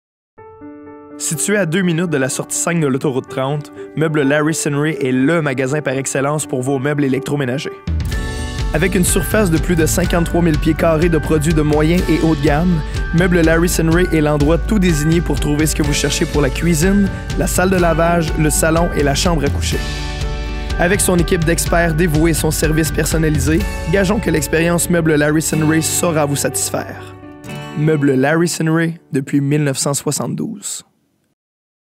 Sinray - vidéo d’entreprise
Meubles Larry Sinray ltée vous accueille dans sa vaste salle de montre située dans la municipalité de Salaberry-de-Valleyfield. Notre entreprise familiale offre un service professionnel, personnalisé et digne de confiance.